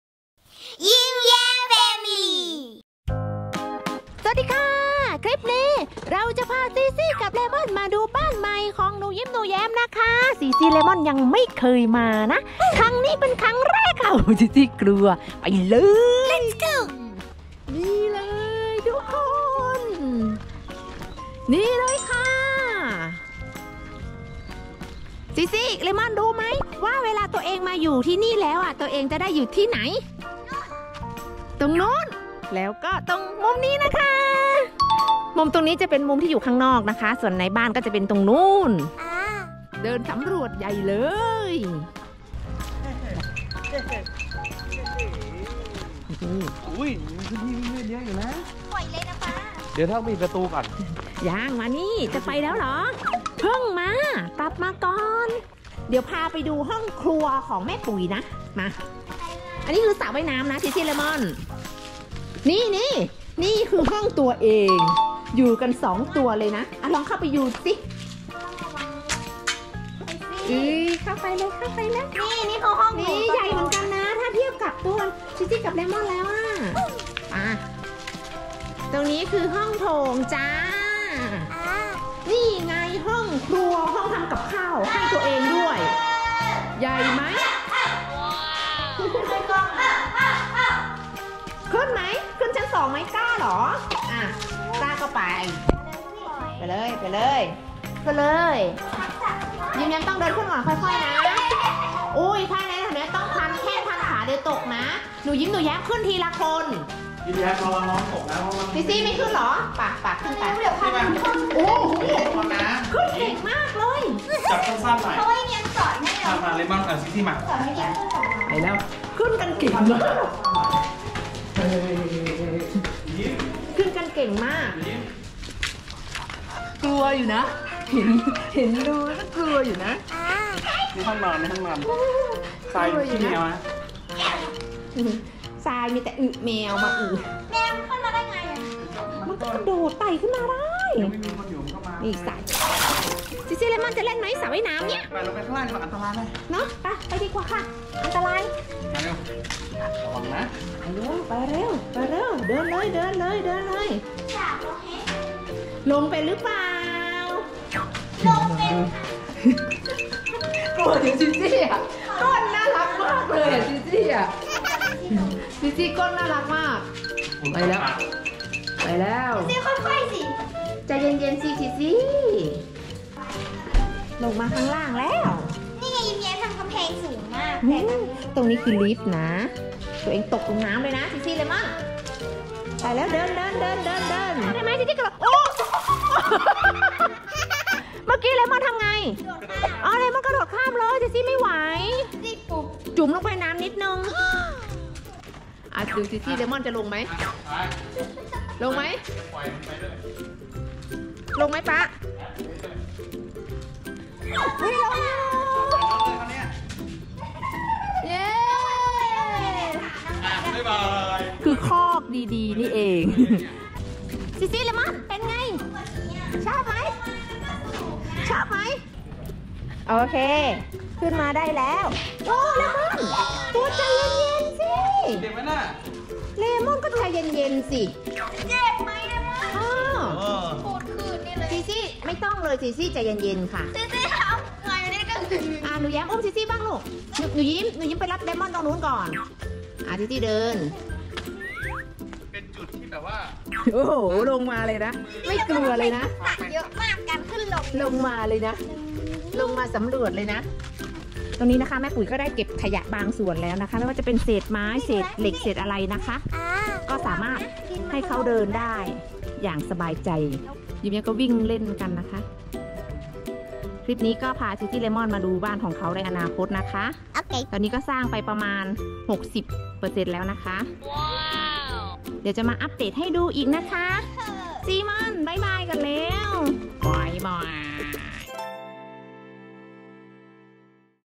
YimYamFamily | พาซีซี่เลม่อนเที่ยวบ้านใหม่ของหนูยิ้มหนูแย้ม
ครั้งแรกที่หนูยิ้มหนูแย้มพาซีซี่เลม่อนมาดูบ้านของตัวเองในอนาคต ซีซี่เลม่อนดูตื่นตาตื่นใจและชอบที่นี่กันสุดๆ เลย คงต้องพามาอีกบ่อยๆ แล้วแหละ